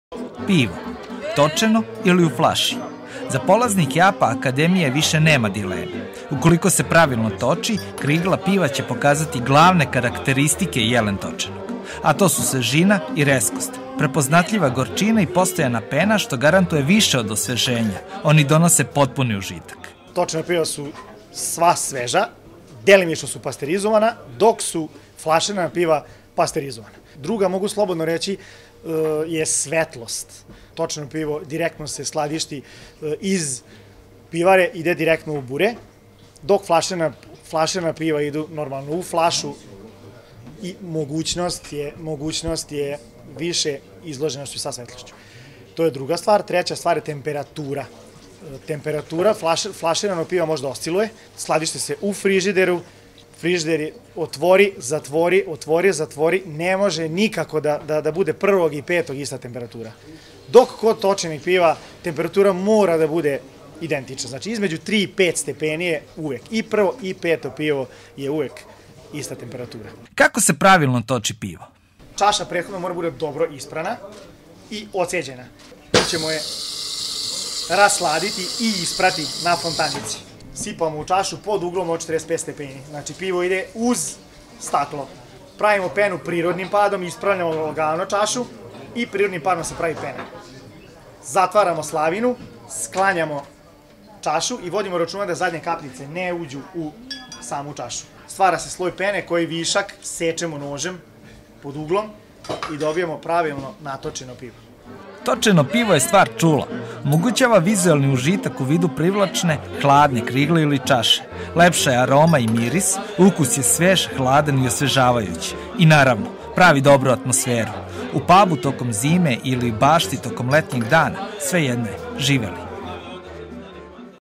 Kako se pravilno toči pivo?
Kako dolazi lepše vreme pivo je sve češće osveženje kod kuće ili u kafiću. Potražili smo odgovor Šta je bolje flasirano ili točeno?